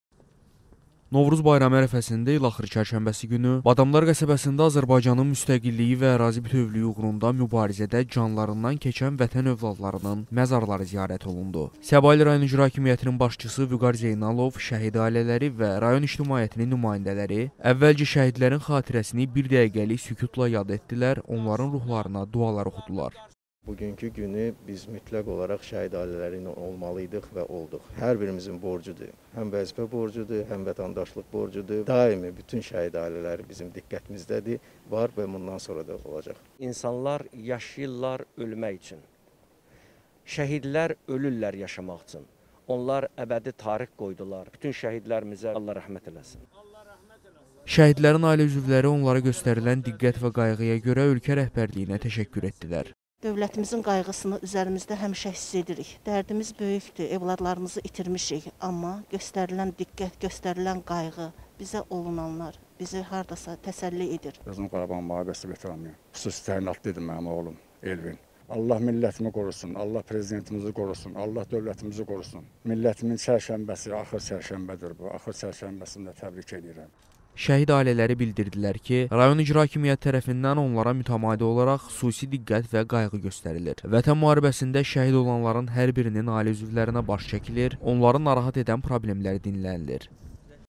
Şəhidlərin məzarı ziyarət edildi
İlaxır çərşənbədə şəhid ailələri övladlarının məzarı başında oldular